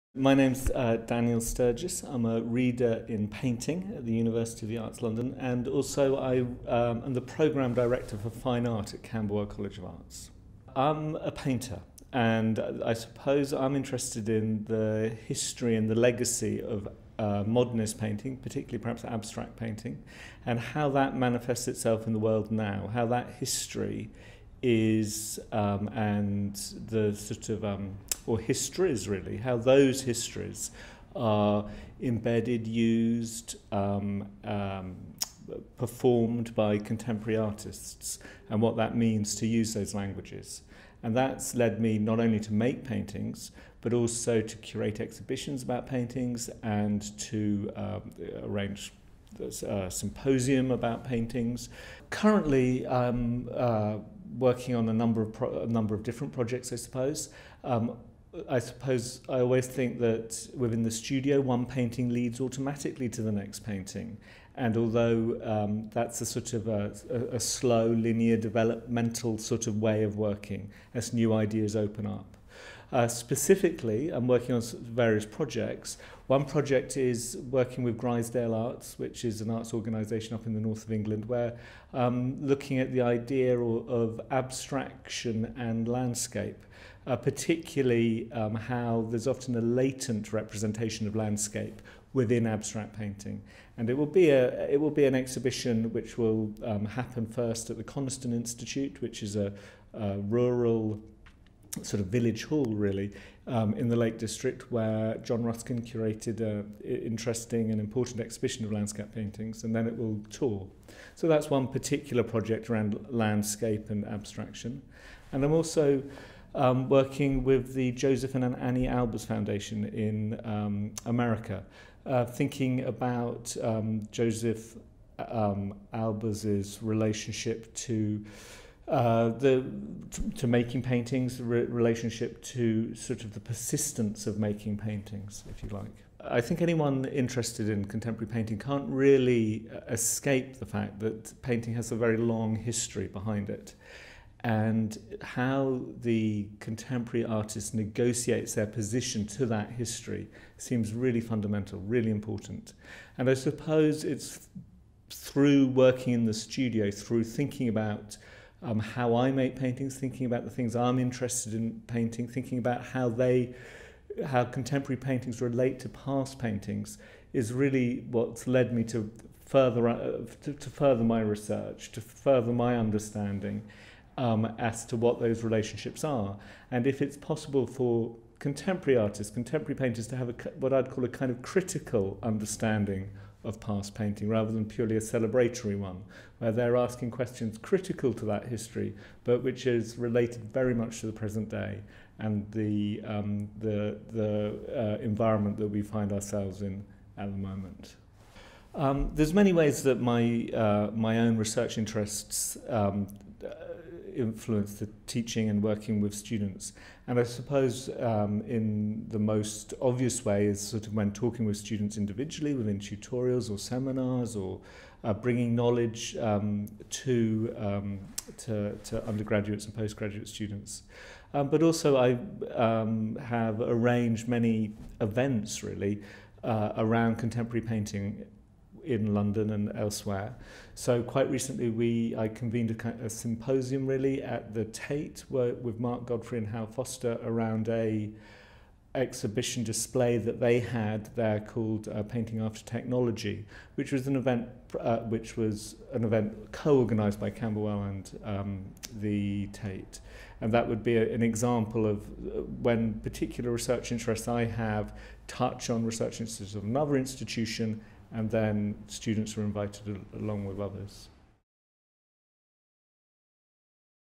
Research Staff Profile: Daniel Sturgis
Daniel Sturgis is an artist and curator whose idiosyncratic, painterly language infuses formalism’s rigorous traditions of visual intellect and craftsmanship with contemporary pop references and casual aesthetics.

His work proposes new possibilities for the continued relevance and vitality of painting and his off-beat geometric arrangements reinvest the optimistic and visionary qualities of modernism in contemporary compositions. Humorous, media savvy and hierarchically democratic, Sturgis’ works advance the legacy of 20th-century abstraction for 21st-century values and sensibilities.

Sturgis’ work is regularly exhibited in the UK and internationally, featuring at museums such as The Chinati Foundation (Marfa, Texas), Camden Art Centre (London) and Turner Contemporary (Margate).

His curated projects include The Indiscipline of Painting (Tate), Daniel Buren’s Voile Toile / Toile Voile (Wordsworth Trust) and Jeremy Moon’s A Retrospective (Kettle’s Yard).

Sturgis is an editor of the Journal of Contemporary Painting and a founding member of Paint Club.

He is a writer and selector for Phaidon’s survey of global painting Vitamin P3. He was a Visiting Professor at Nagoya University of Arts, Japan in 2012, and Visiting Professor in Painting Xi' an Academy of Fine Arts, China in 2015

I am interested in how the culture and language of painting is historically framed and how that history impacts on the contemporary practioner and indeed the practice and culture of painting. Within my own work, my paintings respect and believe in the tenets of modernist abstraction, but also recognise their fallibility. They neither re-stage the arguments and developments of modernist painting, nor revel in post-modern irony. They can be seen to speak with a divided mind.